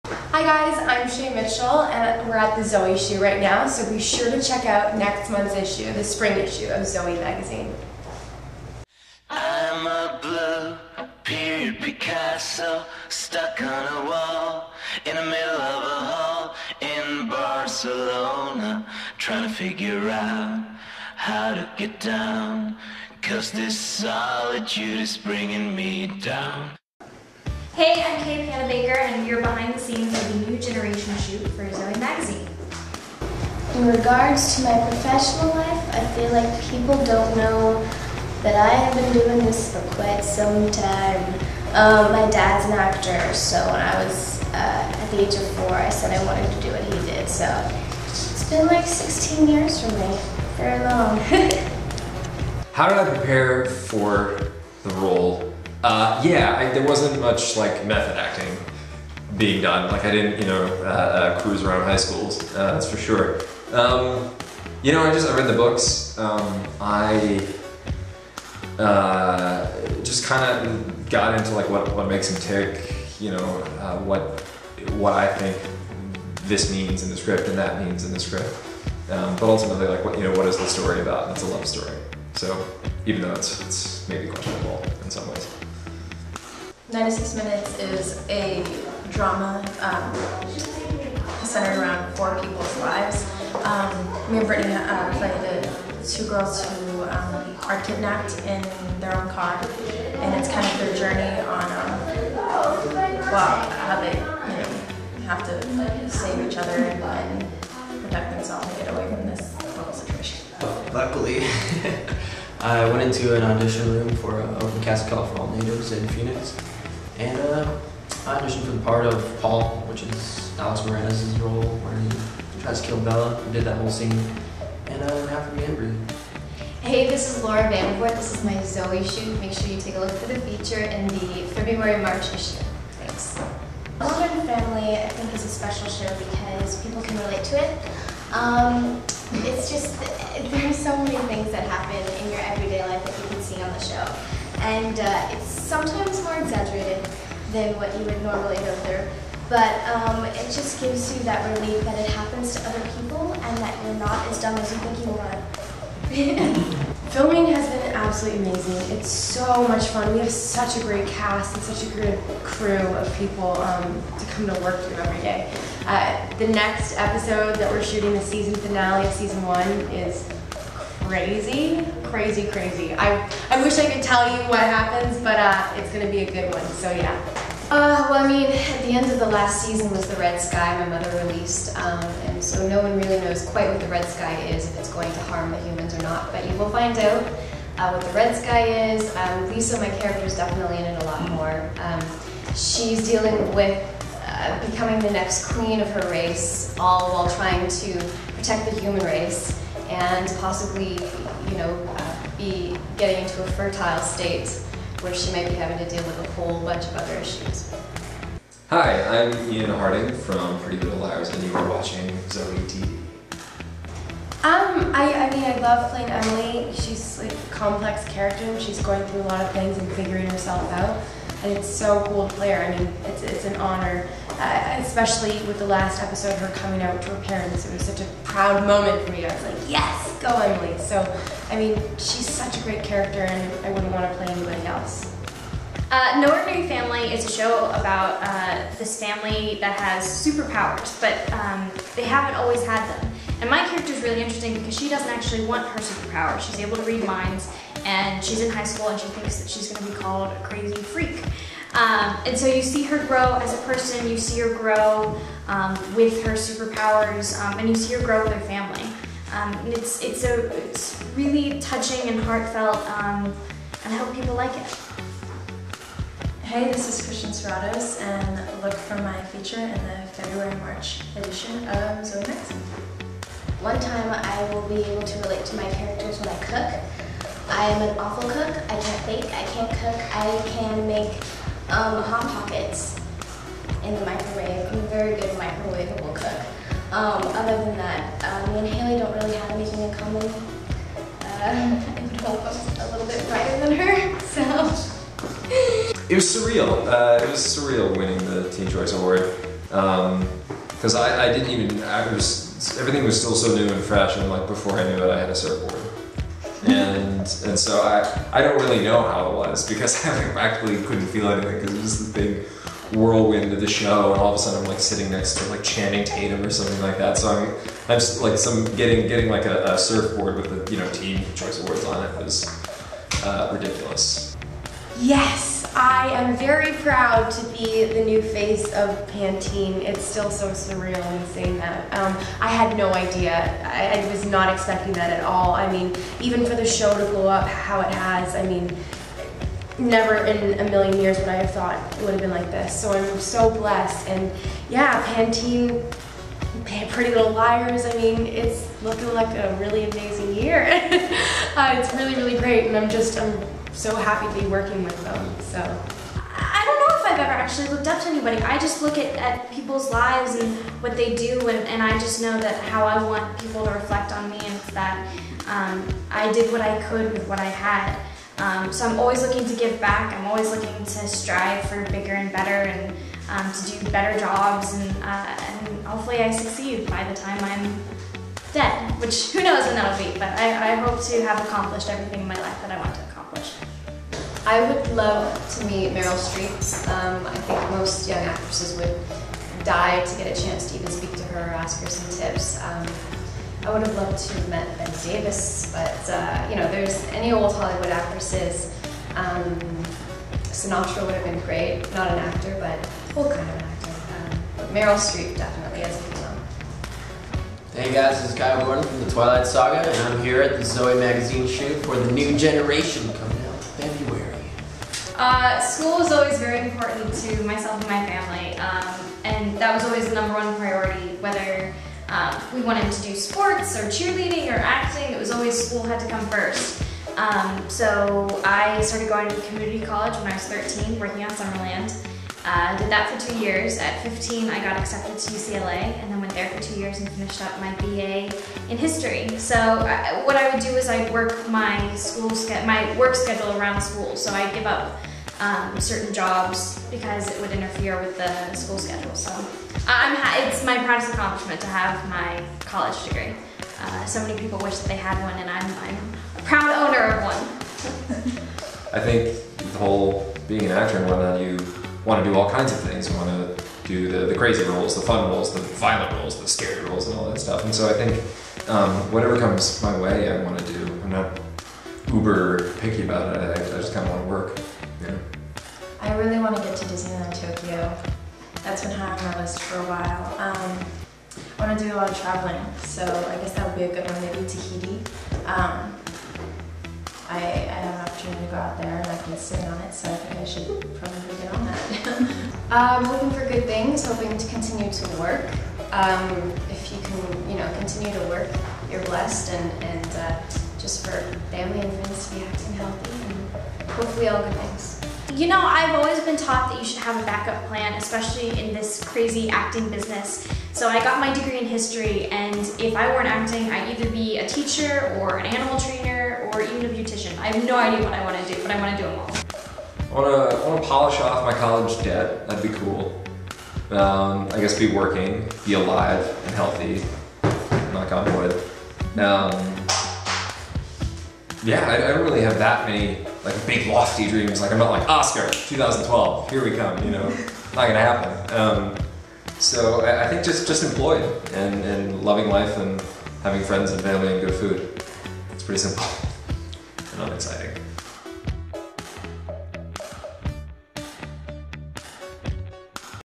INTERVIEWS: NEW GENERATION
Zooey Magazine presents Shay Mitchell, Sarah Hyland, Ian Harding, Laura Vandervoort, Dan Byrd, Kay Panabaker, Kiowa Gordon, and Cristian Serratos.

Videographed by Amanda Elkins